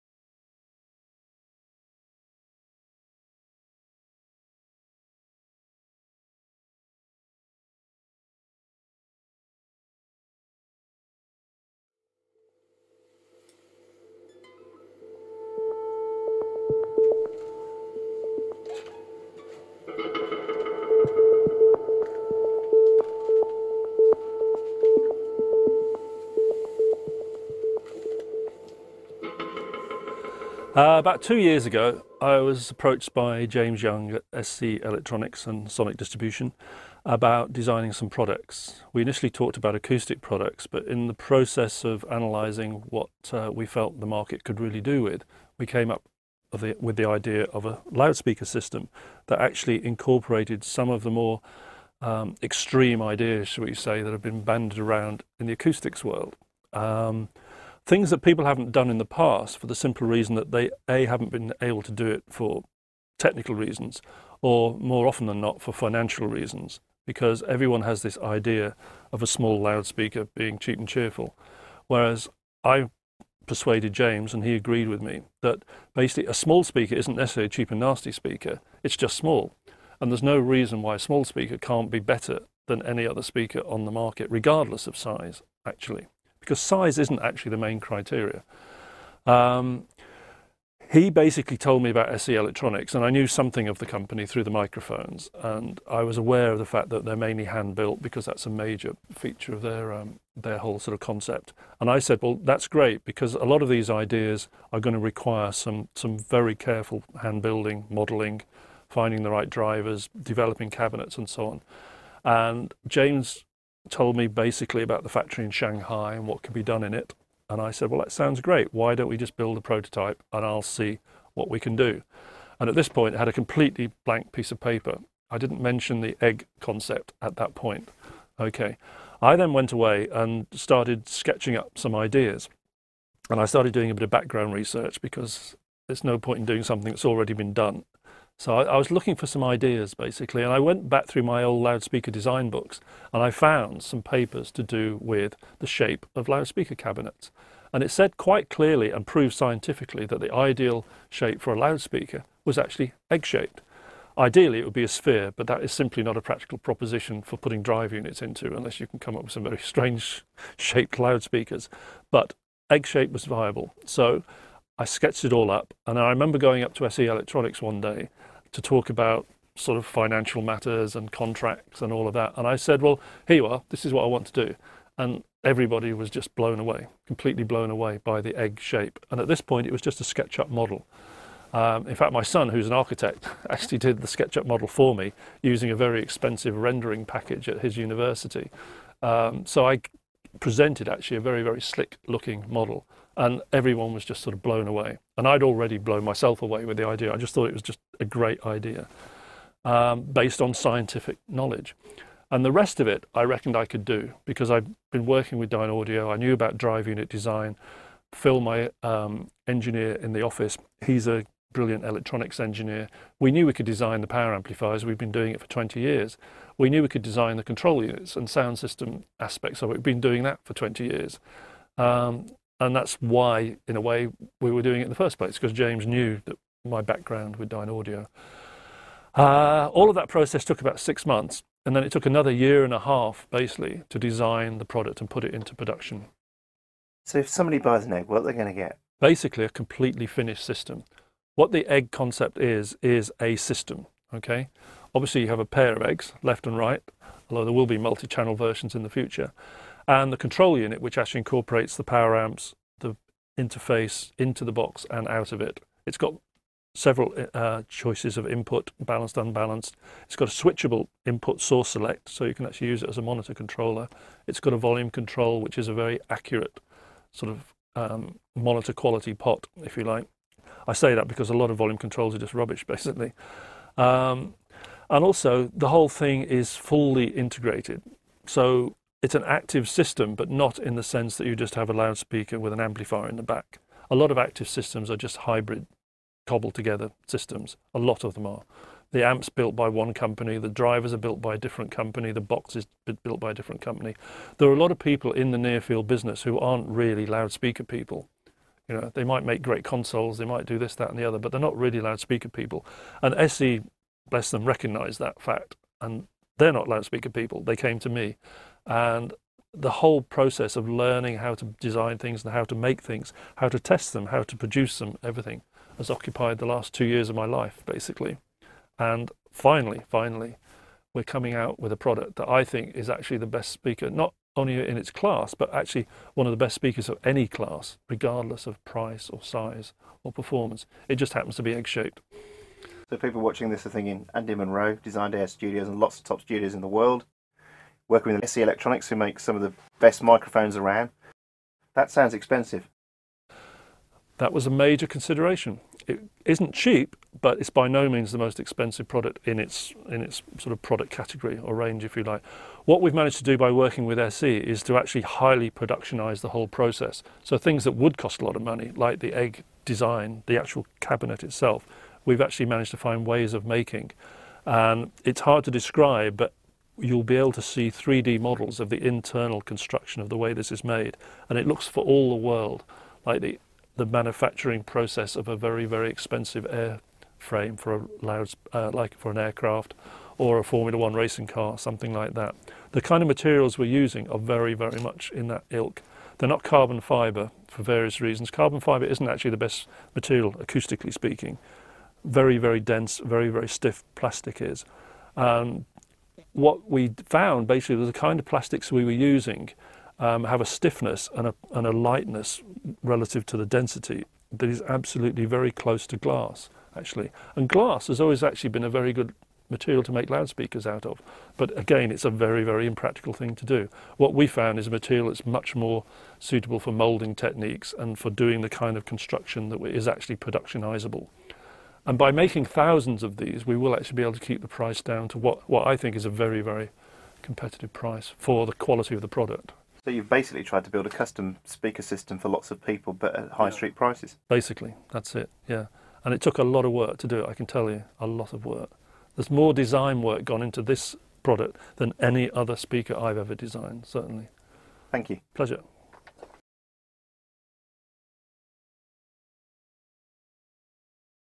Andy Munro sE Munro Egg Monitor - A Revolution In Monitor Design
Andy Munro is a name associated with the sound of some of the world's top studios as an acoustic consultant with 30 years of experience of studio and monitor design. In this video, Andy talks about the development of the sE Munro Egg studio monitors designed in collaboration with sE Electronics's James Ishmaev-Young and Siwei Zou.